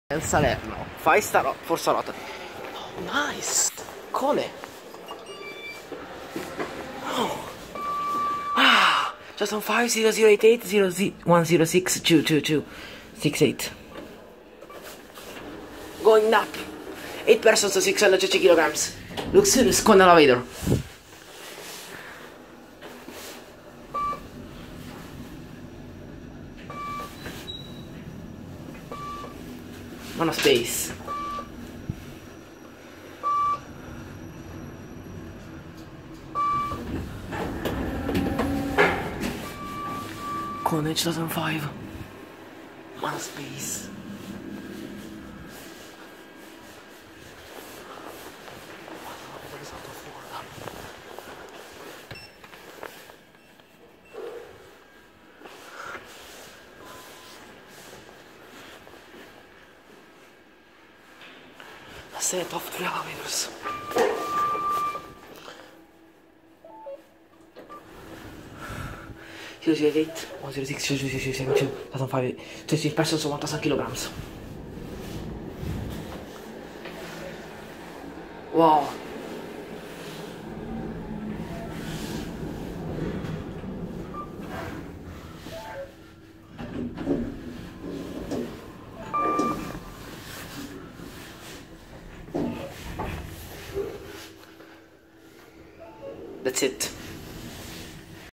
5 2005 Kone Monospace & Minispace Traction Elevators@G**** Hotel Salerno, Salerno, Italy
ADDRESS:Grand Hotel Salerno, Lungomare Clemente Tafuri 1, Salerno, Naples, Italy
NOTE2:I had not much time here in this hotel... there were many security guards...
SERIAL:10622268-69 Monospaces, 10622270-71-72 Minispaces
YEAR:2005
TYPE:MRL Traction Monospaces, Gearless Traction Minispaces
FLOORS SERVED:-2,-1,0,1,2,3,4,5 Monospaces
BRAND:Kone
MODEL:Monospace 700 and Minispace
FIXTURES:KSS 520
POSITION MACHINE:Above, in the shaft and above 5th floor
RECORDED:10/2/18
CAMERA:Samsung Galaxy S7 Edge